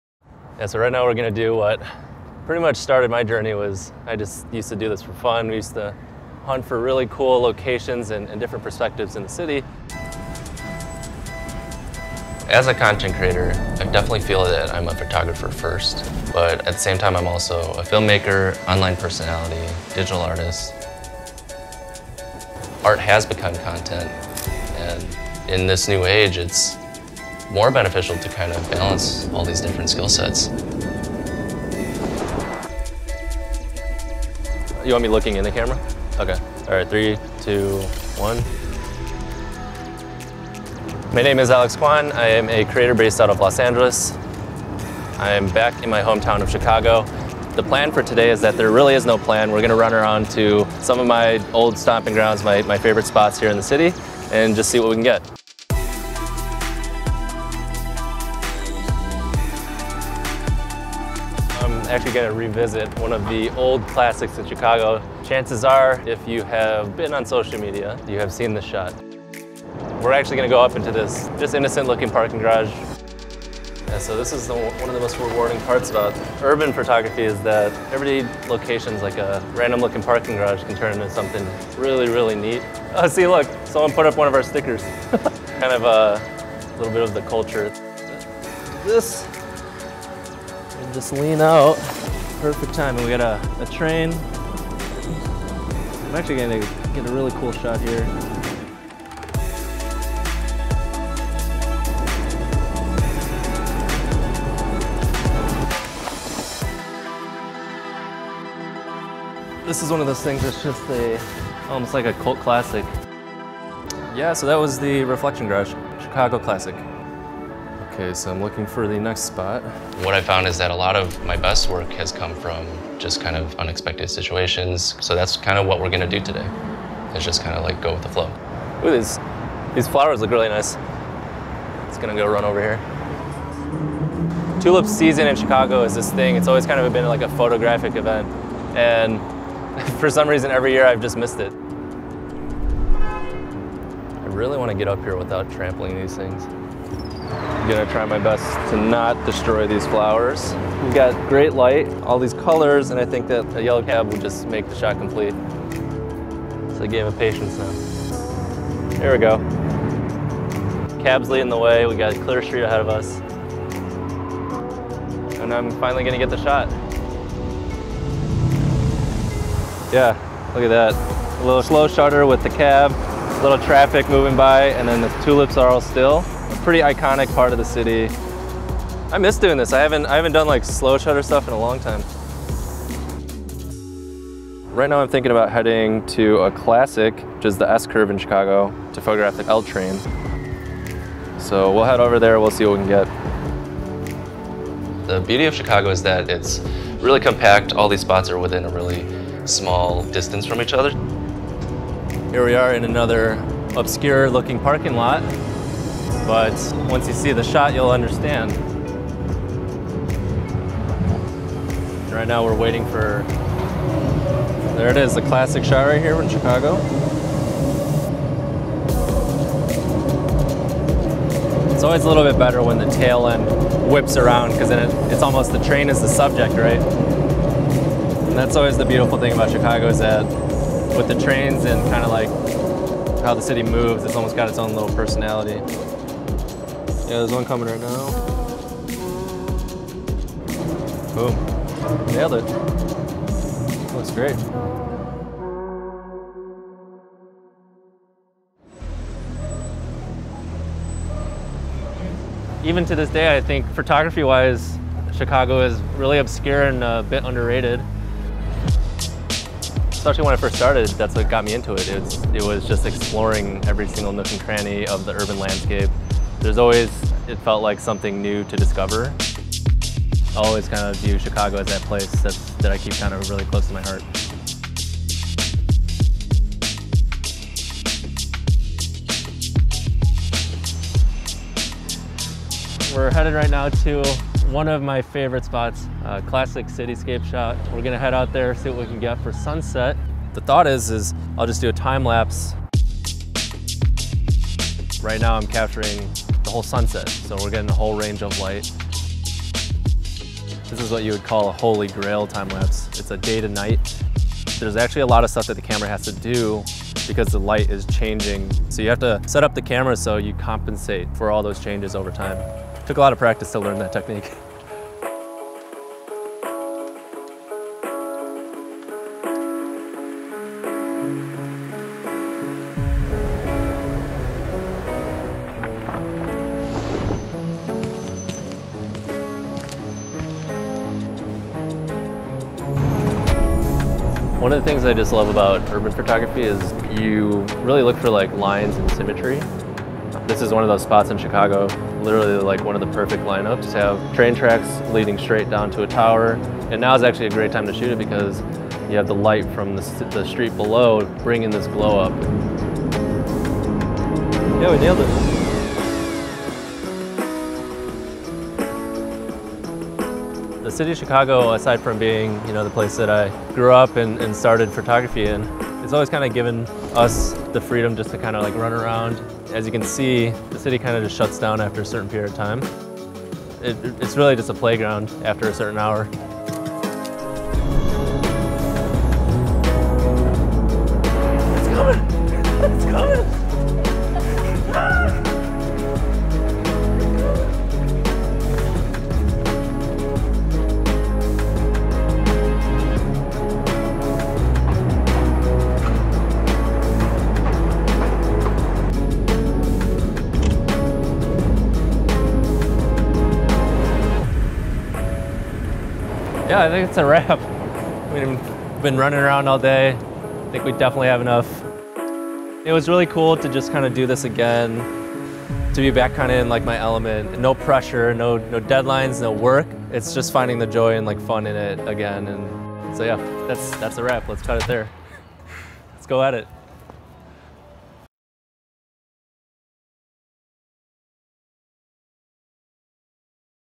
One Day One Artist: Alex Qian
No two days are alike. Every time the sun rises, we get new opportunities to create and grow.

One Day One Artist follows professional photographers while they work, in hopes of seizing the opportunity of a single day.

Our third film in the series follows content creator Alex Qian as he returns to his old stomping grounds—Chicago, the Windy City—in search of favorite spots and unexpected shots.